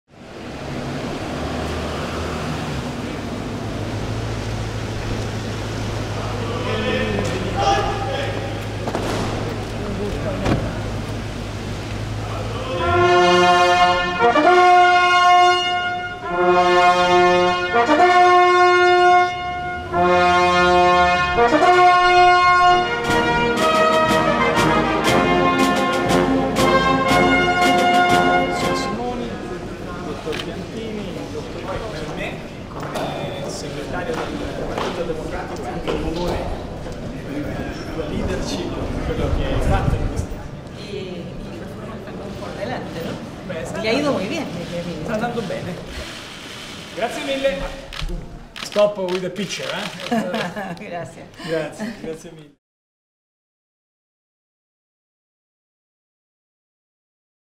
Arrivo a Palazzo Chigi del Presidente della Repubblica del Cile, Michelle Bachelet
Il Presidente del Consiglio Matteo Renzi riceve a Palazzo Chigi il Presidente della Repubblica del Cile, Michelle Bachelet.